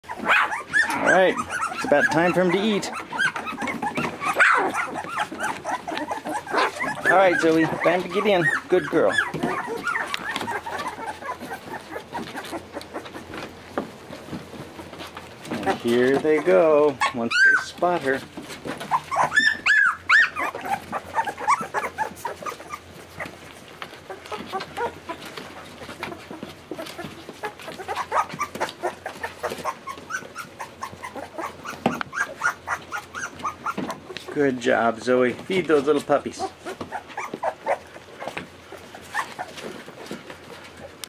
Doodle Puppy Feeding Time!
The Goldendoodle puppies may be only 16 days old, but their appetite is constantly increasing - in fact, these Goldendoodles in San Diego do pretty much three things now - Sleep, Eat, and...well...you can figure out the third.
Puppies can go to their new homes on July 18 - info